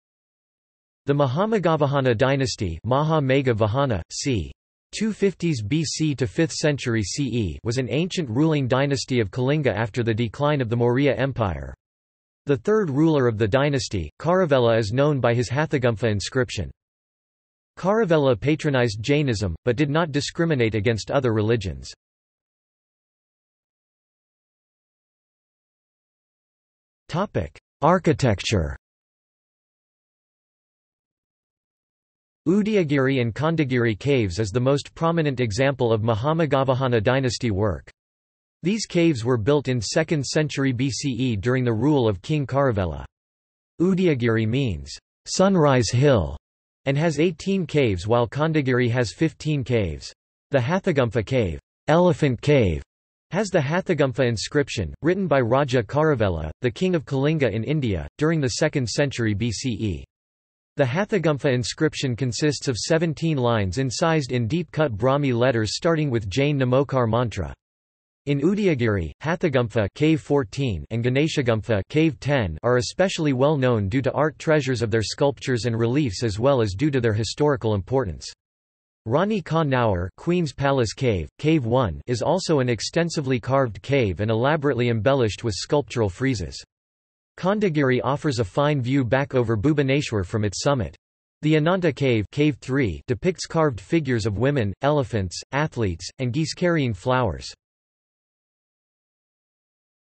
Mahameghavahana dynasty | Wikipedia audio article
This is an audio version of the Wikipedia Article:
Mahameghavahana dynasty

00:00:23 undefined

- Socrates

SUMMARY
The Mahameghavahana dynasty (Mahā-Mēgha-Vāhana, c. 250s BC to 5th century CE) was an ancient ruling dynasty of Kalinga after the decline of the Maurya Empire. The third ruler of the dynasty, Kharavela is known by his Hathigumpha inscription.
Kharavela patronised Jainism, but did not discriminate against other religions.